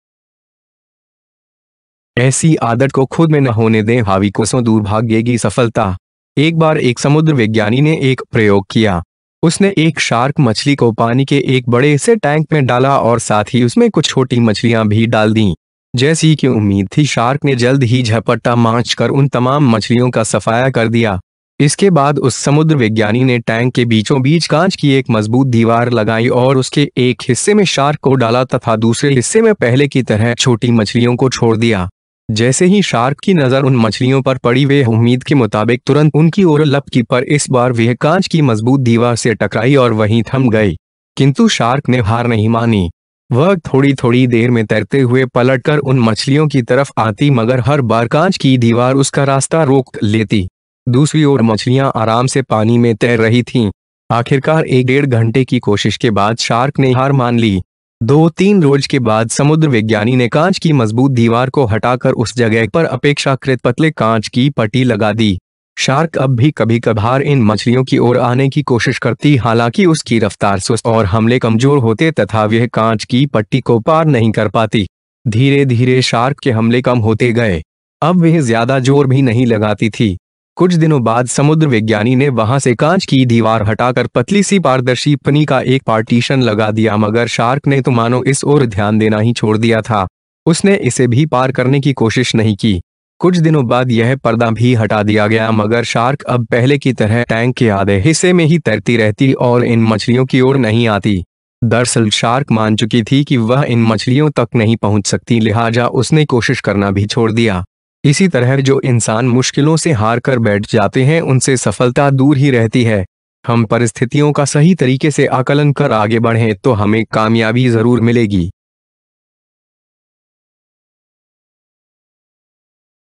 ऐसी आदत को खुद में न होने दें हावी, कोसों दूर भागेगी सफलता
एक बार एक समुद्र विज्ञानी ने एक प्रयोग किया। उसने एक शार्क मछली को पानी के एक बड़े-से टैंक में डाला और साथ ही उसमें कुछ छोटी मछलियां भी डाल दीं।
जैसी कि उम्मीद थी, शार्क ने जल्द ही झपट्टा मार कर उन तमाम मछलियों का सफाया कर दिया। इसके बाद उस समुद्र विज्ञानी ने टैंक के बीचों-बीच कांच की एक मजबूत दीवार लगाई और उसके एक हिस्से में शार्क को डाला तथा दूसरे हिस्से में पहले की तरह छोटी मछलियों को छोड़ दिया। जैसे ही शार्क की नजर उन मछलियों पर पड़ी, वह उम्मीद के मुताबिक तुरंत उनकी ओर लपकी, पर इस बार वह कांच की मजबूत दीवार से टकराई और वहीं थम गई। किंतु शार्क ने हार नहीं मानी। वह थोड़ी-थोड़ी देर में तैरते हुए पलटकर उन मछलियों की तरफ आती, मगर हर बार कांच की दीवार उसका रास्ता रोक लेती।

दूसरी ओर मछलियां आराम से पानी में तैर रही थीं। आखिरकार एक-डेढ़ घंटे की कोशिश के बाद शार्क ने हार मान ली। दो-तीन रोज के बाद समुद्र विज्ञानी ने कांच की मजबूत दीवार को हटाकर उस जगह पर अपेक्षाकृत पतले कांच की पट्टी लगा दी। शार्क अब भी कभी-कभार इन मछलियों की ओर आने की कोशिश करती, हालांकि उसकी रफ्तार सुस्त और हमले कमजोर होते तथा वह कांच की पट्टी को पार नहीं कर पाती।

धीरे-धीरे शार्क के हमले कम होते गए। अब वह ज्यादा जोर भी नहीं लगाती थी। कुछ दिनों बाद समुद्र विज्ञानी ने वहां से कांच की दीवार हटाकर पतली-सी पारदर्शी पन्नी का एक पार्टीशन लगा दिया, मगर शार्क ने तो मानो इस ओर ध्यान देना ही छोड़ दिया था।
उसने इसे भी पार करने की कोशिश नहीं की। कुछ दिनों बाद यह पर्दा भी हटा दिया गया, मगर शार्क अब पहले की तरह टैंक के आधे हिस्से में ही तैरती रहती और इन मछलियों की ओर नहीं आती।

दरअसल शार्क मान चुकी थी कि वह इन मछलियों तक नहीं पहुंच सकती, लिहाजा उसने कोशिश करना भी छोड़ दिया। इसी तरह जो इंसान मुश्किलों से हारकर बैठ जाते हैं, उनसे सफलता दूर ही रहती है। हम परिस्थितियों का सही तरीके से आकलन कर आगे बढ़ें तो हमें कामयाबी जरूर मिलेगी।